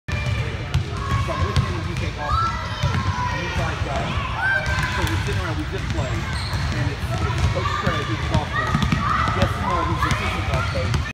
MEYEND Vision + Performance: Dakari Johnson Camp Senaptec Strobe Training Glasses Demo
Demonstration of the appearance for the wearer of the Senaptec Strobe on difficulty level 1. The Senaptec Strobe is designed to train the connections between an individual's eyes, brain, and body. Using liquid crystal technology, the lenses flicker between clear and opaque, removing visual information and forces the individual to process more efficiently. The Senaptec Strobe can be integrated into existing training drills and exercises.